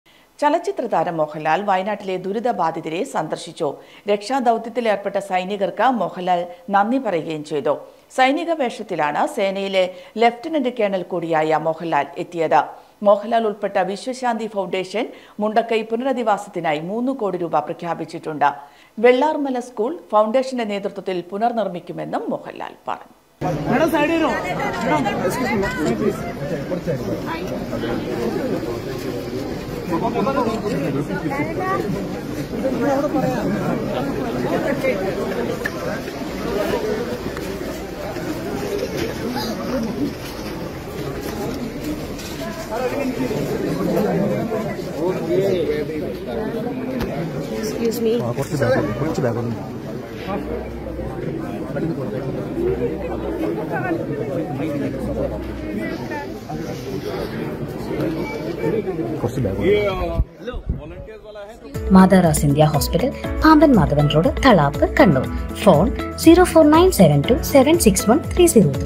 ചലച്ചിത്രതാരം മോഹൻലാൽ വയനാട്ടിലെ ദുരിതബാധിതരെ സന്ദർശിച്ചു.
രക്ഷാദൗത്യത്തിൽ ഏർപ്പെട്ട സൈനികർക്ക് മോഹൻലാൽ നന്ദി പറയുകയും ചെയ്തു..